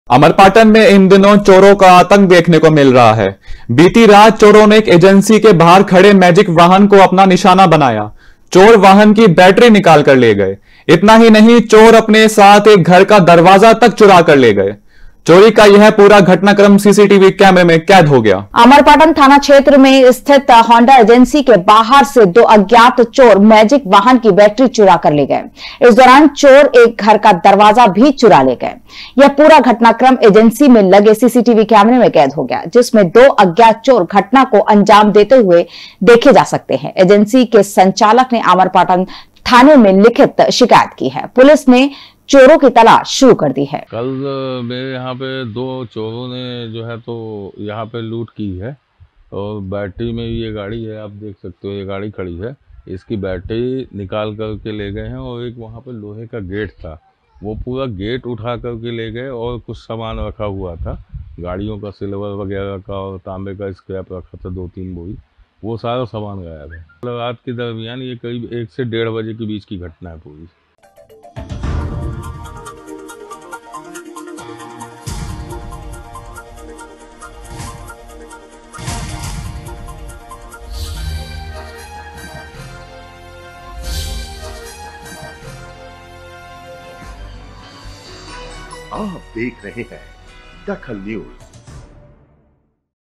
DAKHAL NEWS अमरपाटन थाना क्षेत्र में चोरों का आतंक
अमरपाटन थाना क्षेत्र में चोरों का आतंक
चोरी की घटना सीसीटीवी में हुई कैद